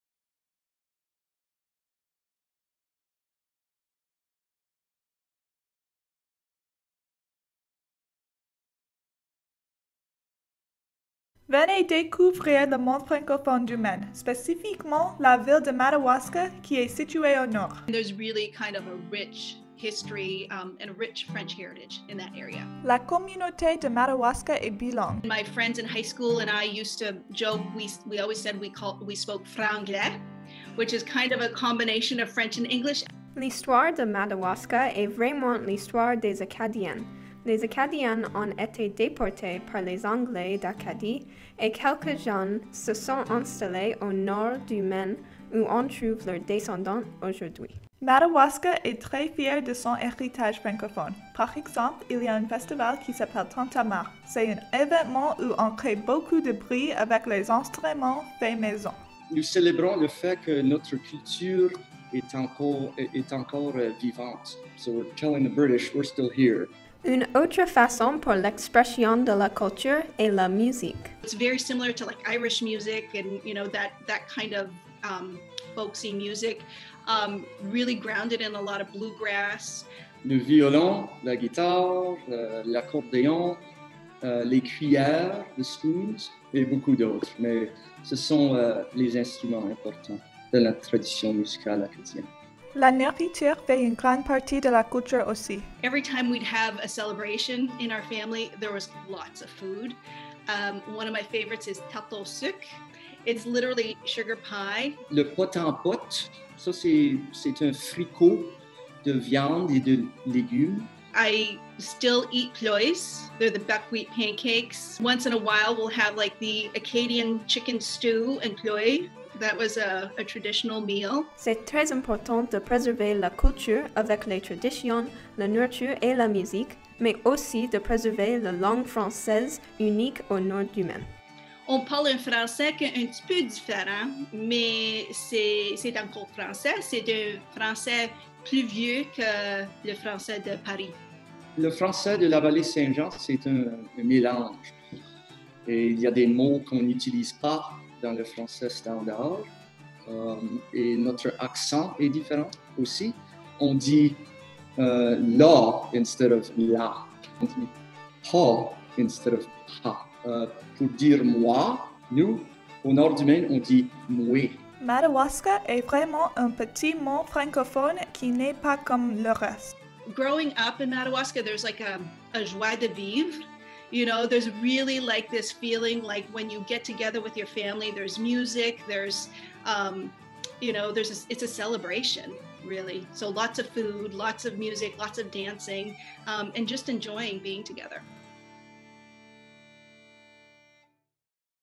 Come discover the French World in Maine
A short documentary by Kathryn Busko and Carolyne Sauda, juniors at Bangor High School, exploring the French World of Madawaska Maine. Kathryn and Carolyne won first place in the 2021 AATF National French Week Media Contest earning the maximum score available out of more than 90 submissions country wide.